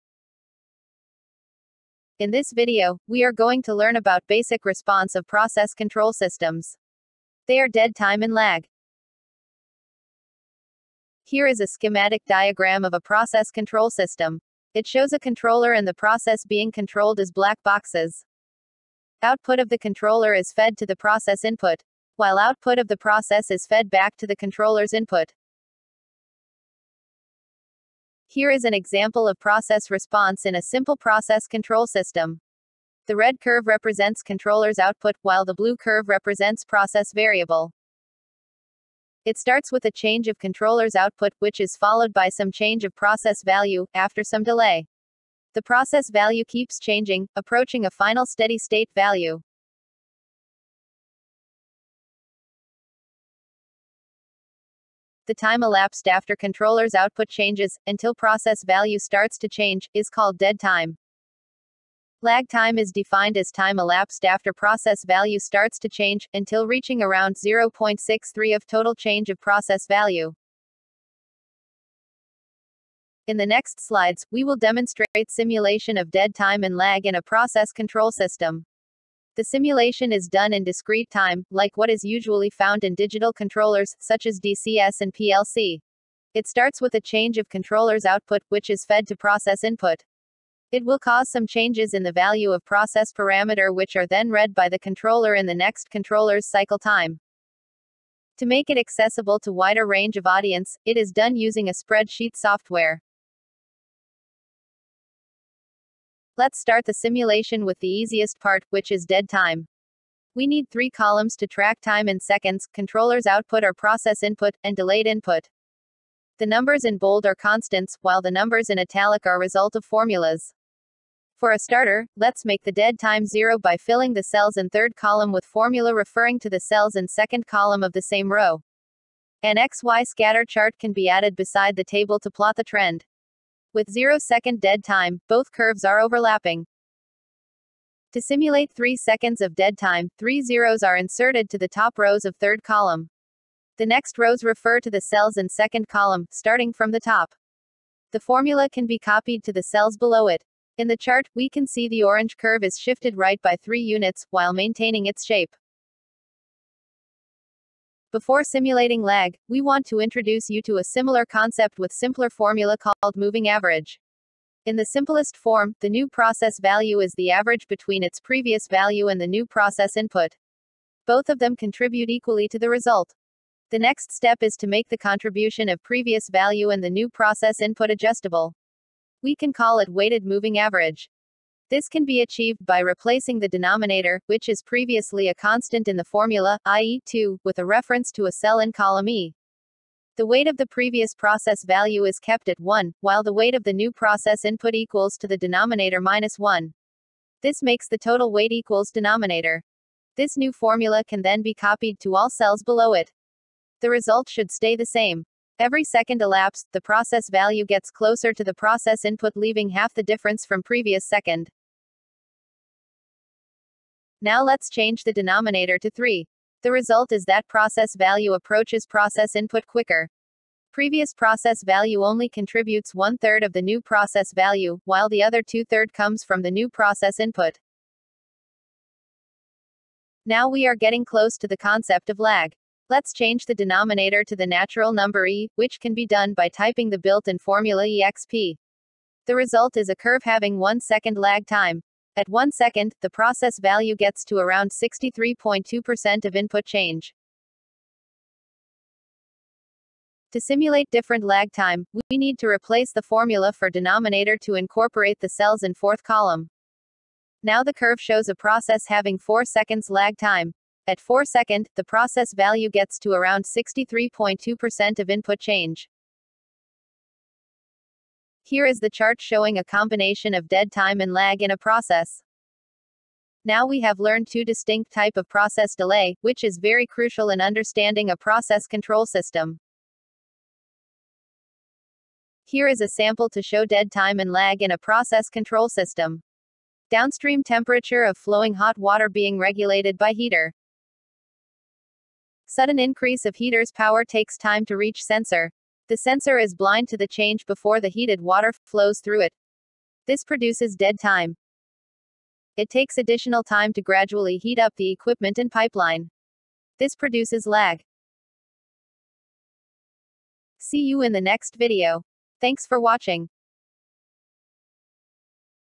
Understanding Process Control System 4 : Dead Time & Lag
Understanding dead time and lag as basic process responses through simulation using spreadsheet.